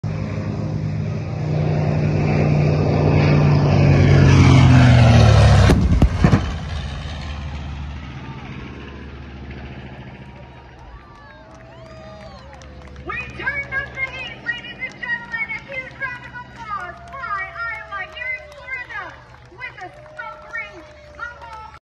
Bombing Run with a B-25 -Fly Iowa-
Witness what a bombing run would like from a B-25J bomber!  Check out this bombing run with the Tora Bomb Squad at the Clarinda, Iowa Air Show.

Video by Eric Herman